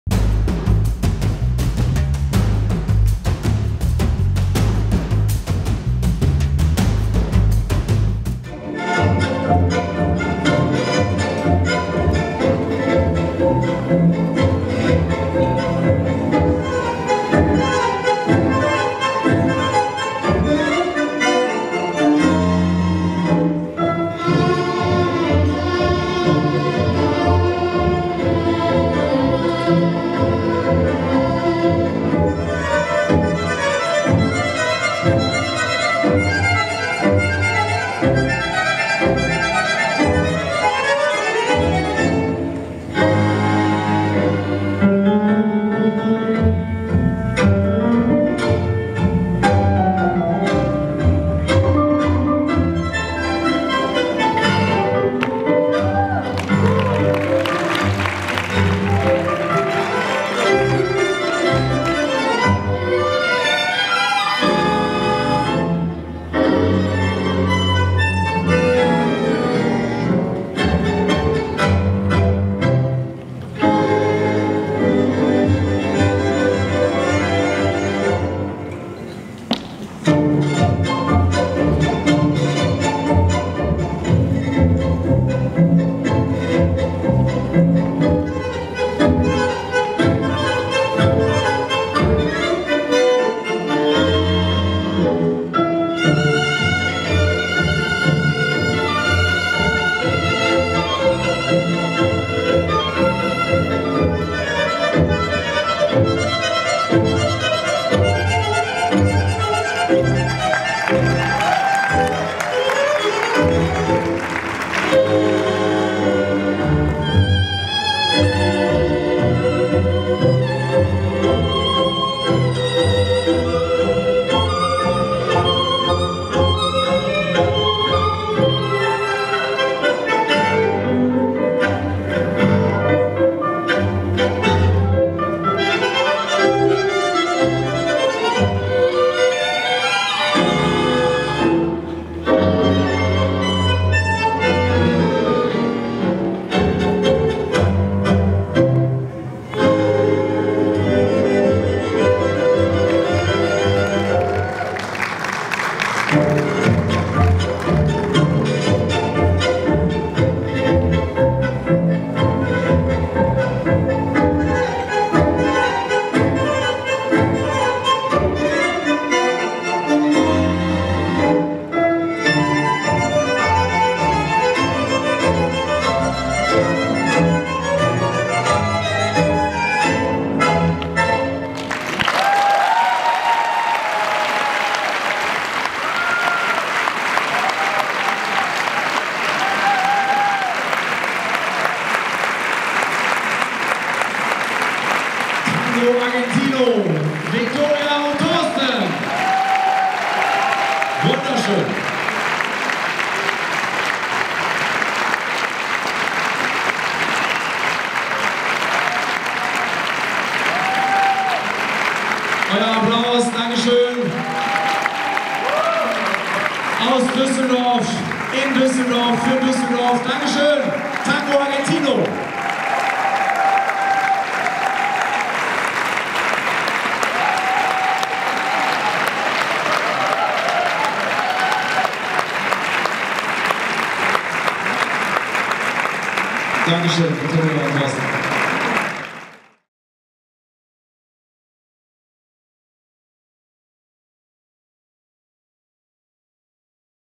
Tortsen Thiele - Viktoria Baumann | DanceStars Gala Düsseldorf 2017 - Tango Argentino Show
DanceStars Gala Düsseldorf 2017, Rendezvous of the World Champions on 3rd of December 2017 at the Maritim Hotel Düsseldorf, presented by Wladislaw Lalafarjan and Roman Frieling.